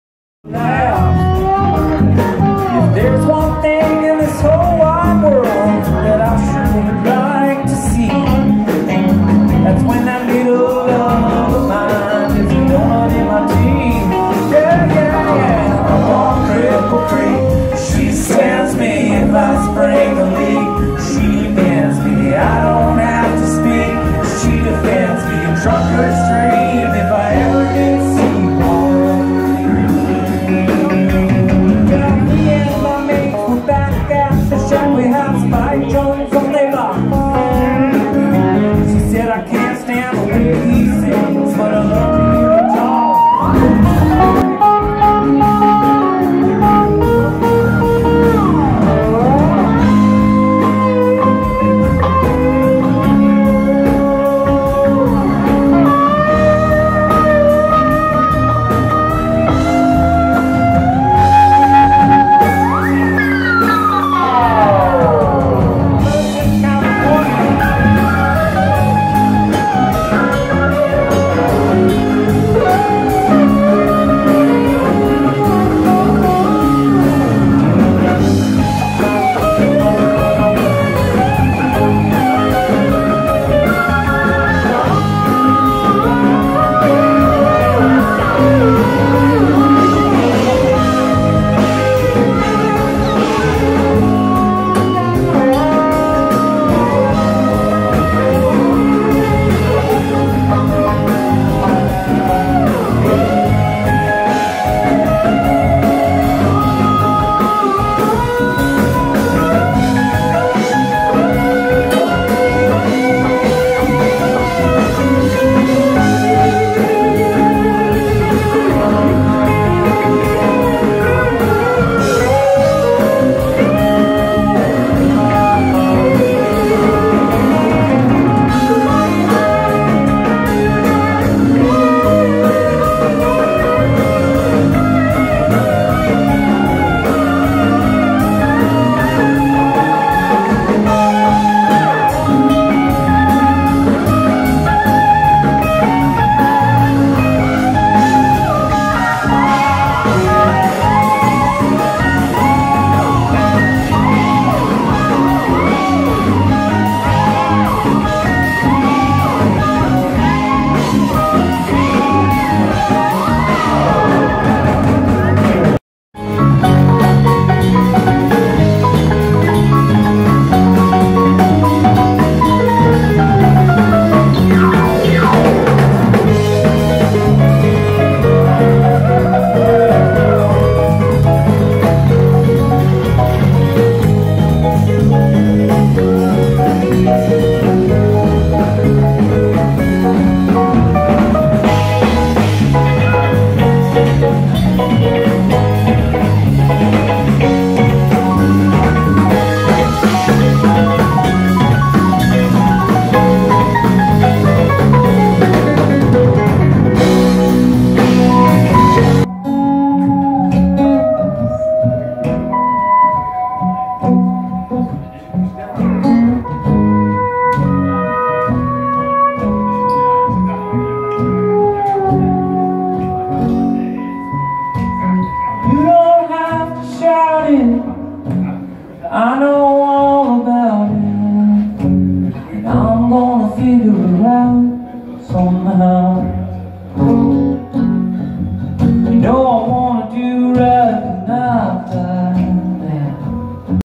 Assembly of Dust, Club Fox, Redwood City, CA
Mashup of clips from Redwood City show, 4/21/2016
Great band in a tiny club.